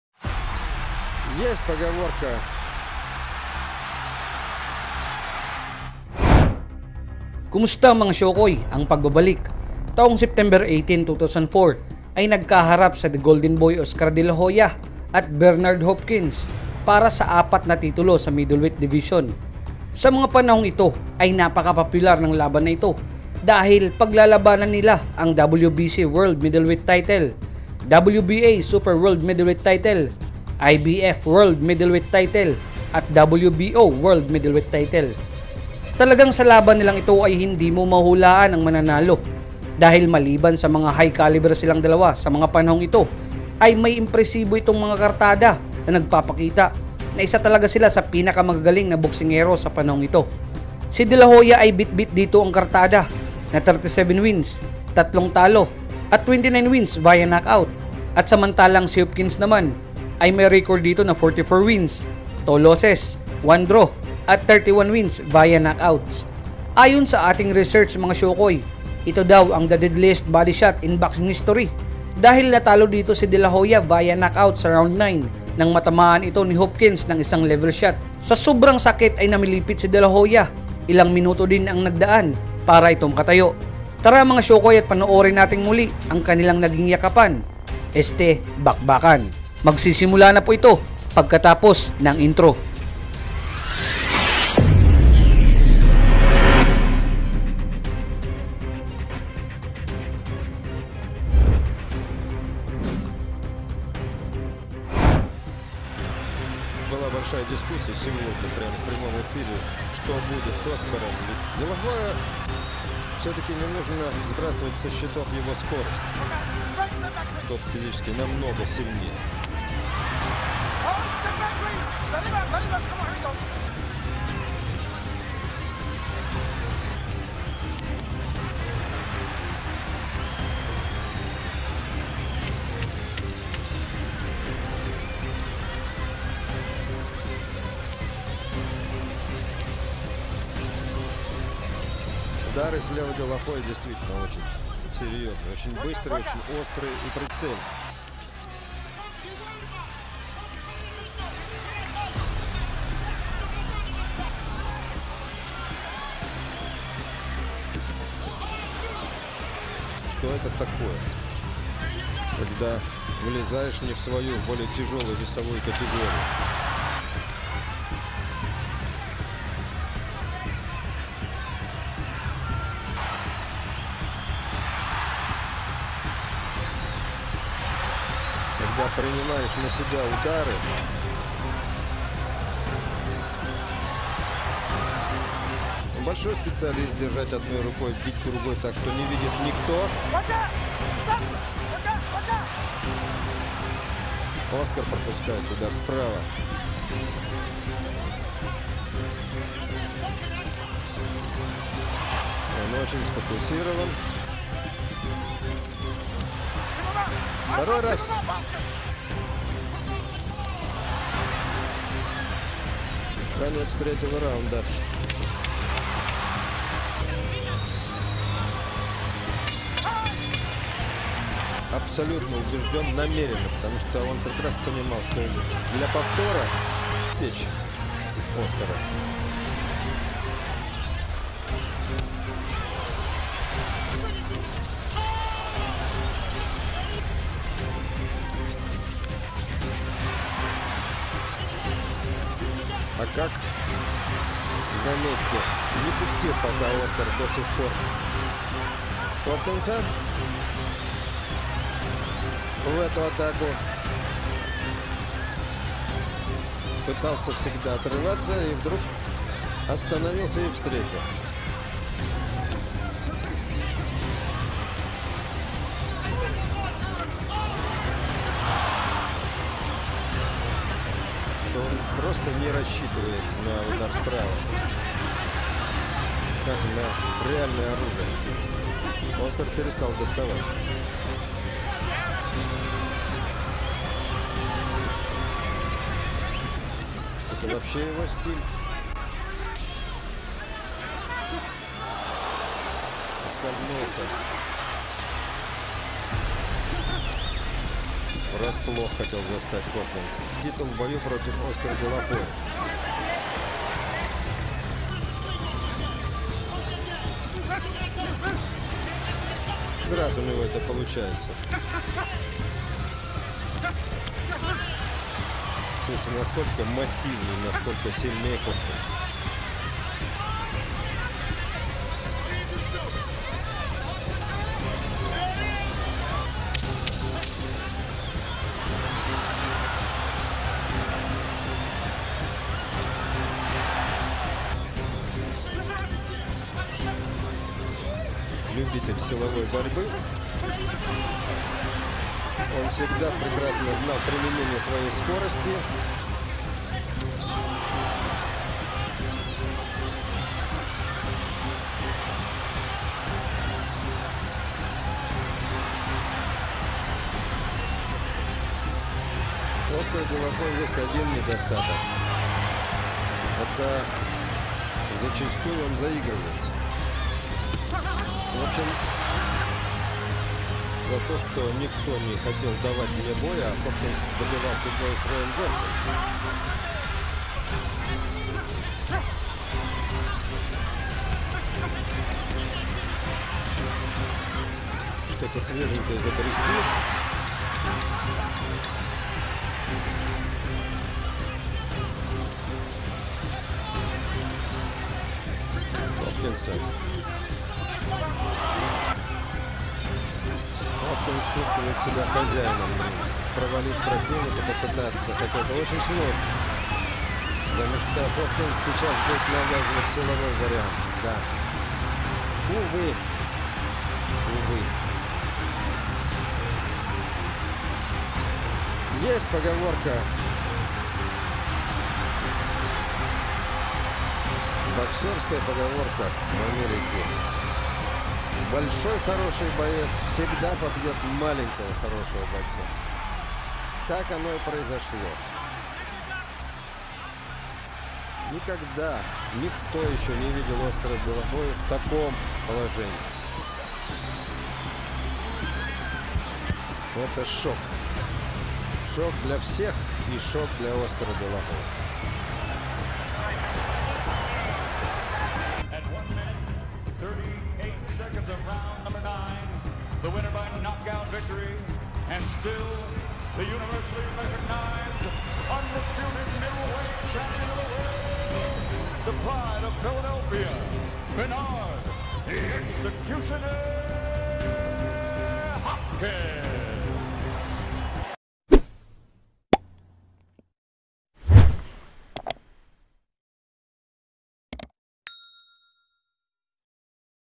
Bru-tal na Body shot ang inabot ni dela hoya | DELA HOYA VS HOPKINS
ANG PINAKA MASAKIT NA BODY SHOT SA KASAYSAYAN NG BOXING | DELA HOYA VS HOPKINS

This is boxing, Oscar Dela Hoya vs Bernard Hopkins full fight highlights. For the WBC World Middle Weight Title, WBA Super World middle weight title, IBF world middle weight title and WBO world middle weight title.